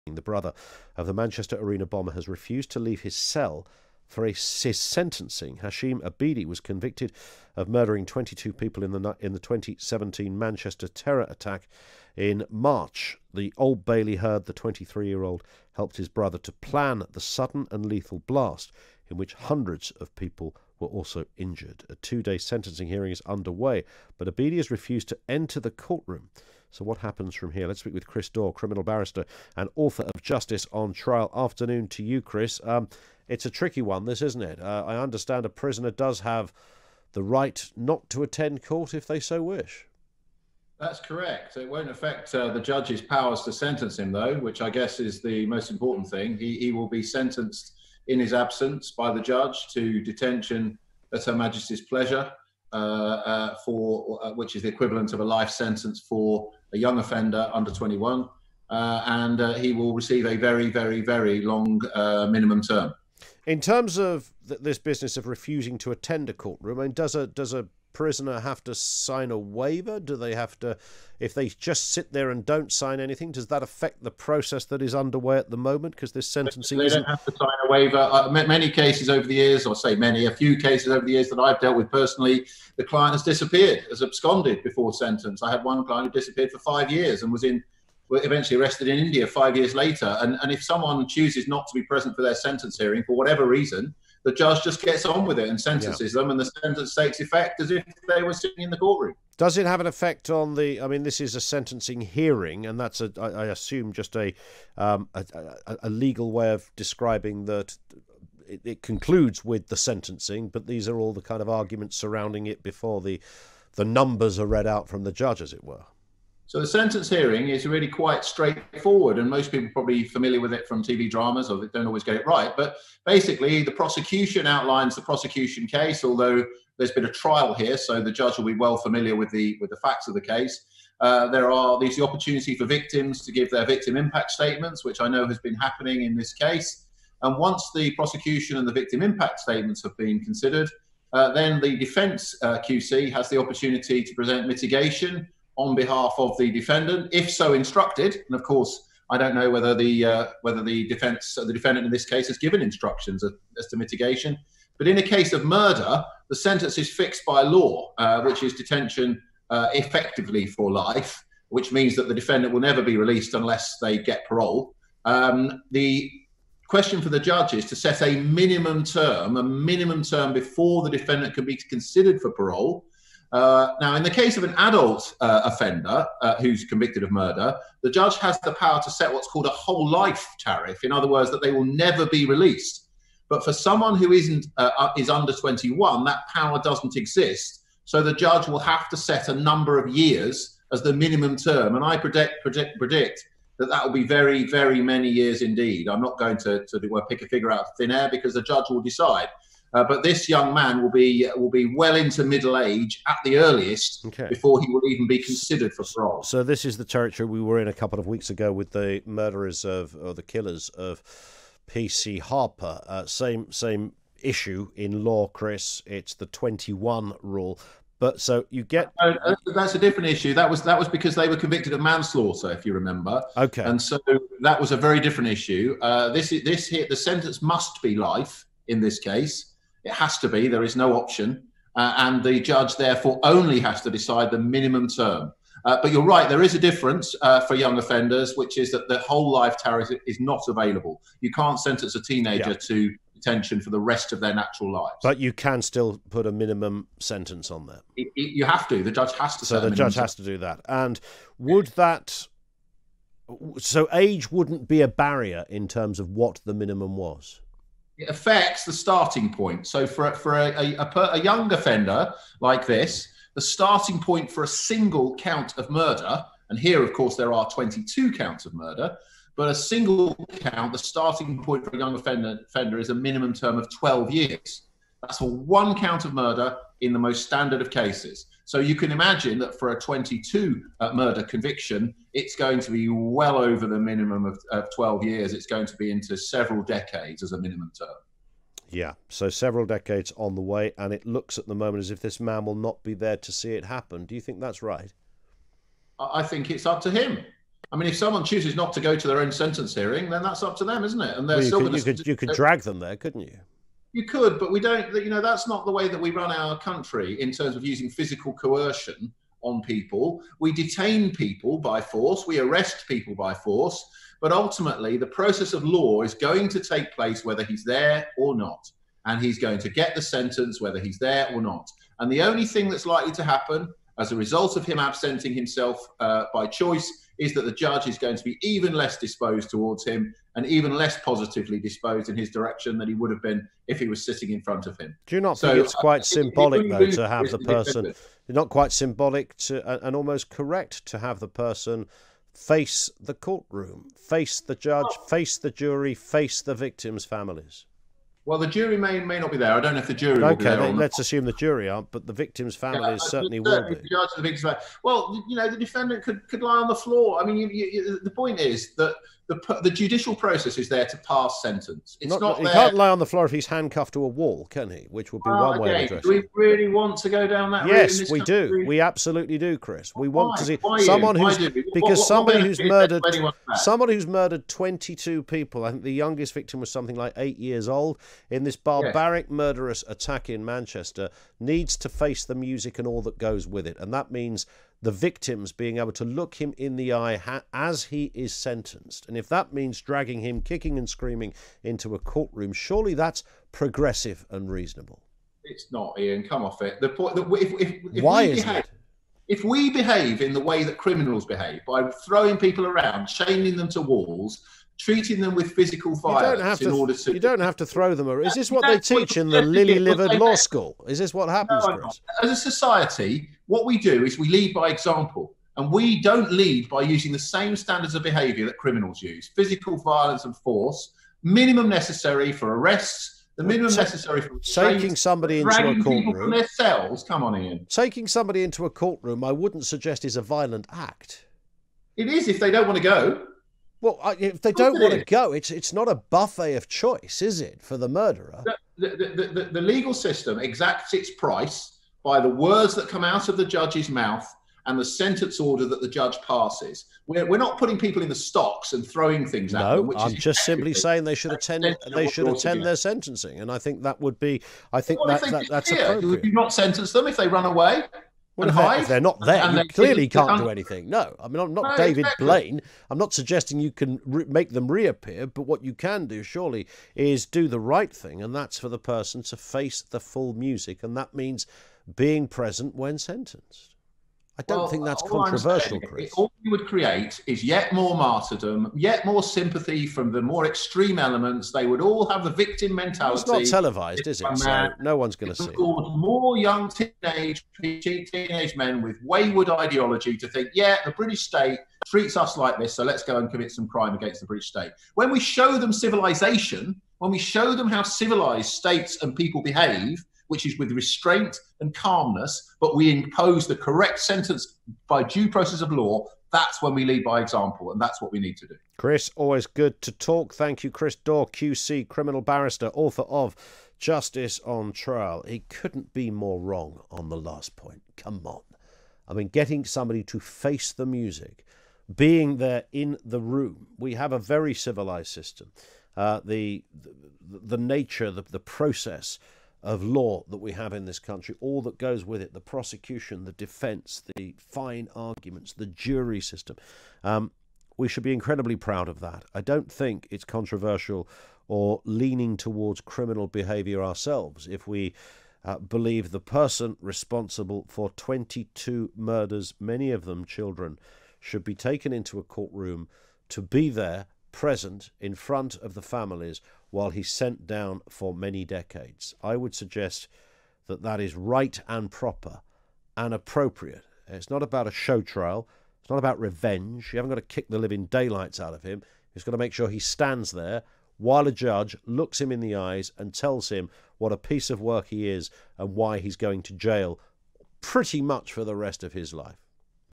Should convicted murderers be forced to attend their sentence hearing?
Ian and criminal barrister Chris Daw QC debate whether the criminals should be forced to attend court to receive their sentences from the judge.

It comes after Hashem Abedi, the brother of the Manchester Arena bomber, refused to attend his sentencing hearing for 22 counts of murder, attempted murder and plotting to cause an explosion likely to endanger life.

Ian: “He needs to face the music and all that goes with it and that means the victims being able to look him in the eye as he is sentenced.”

Chris: “All you would create is yet more martyrdom, yet more sympathy from the more extreme elements, they would all have the victim mentality.”